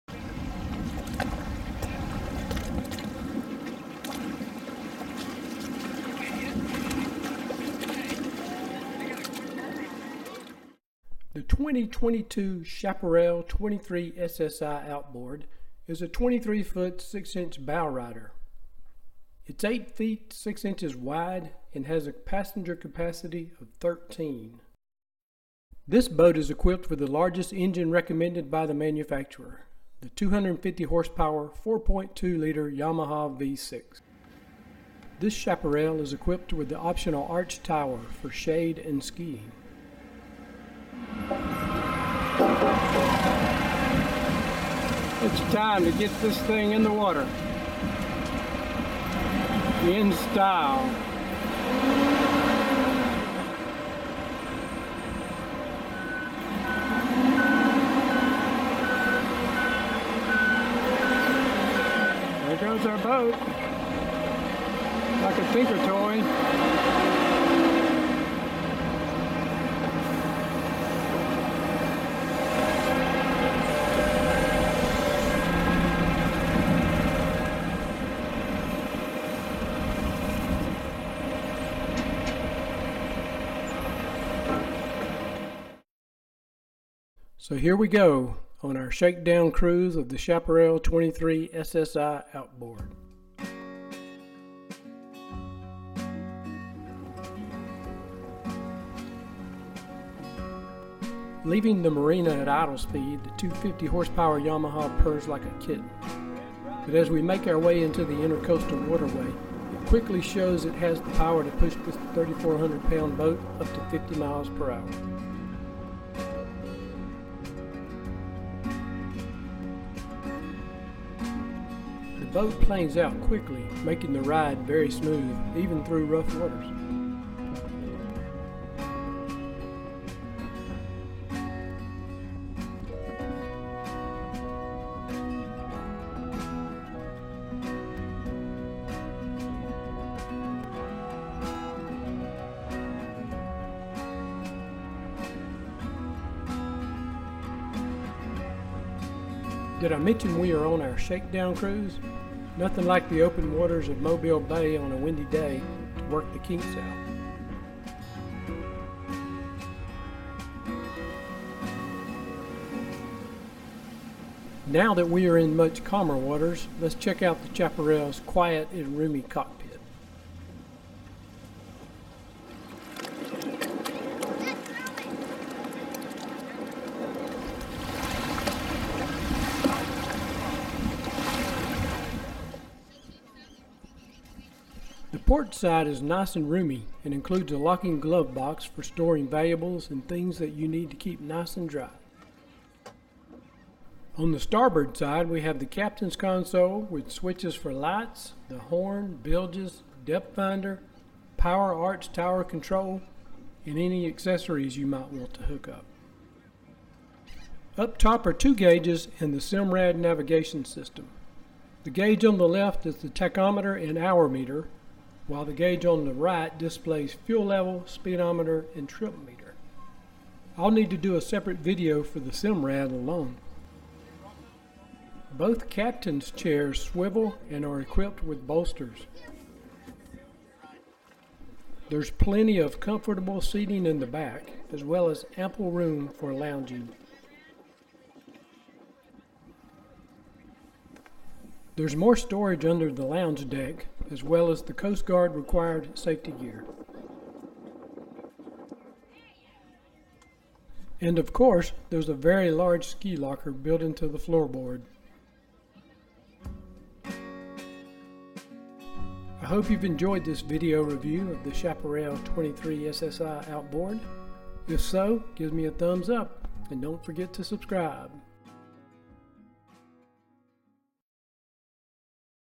Chaparral 23SSI-OB Video Review
A on the water review of the Chaparral 23SSI-OB.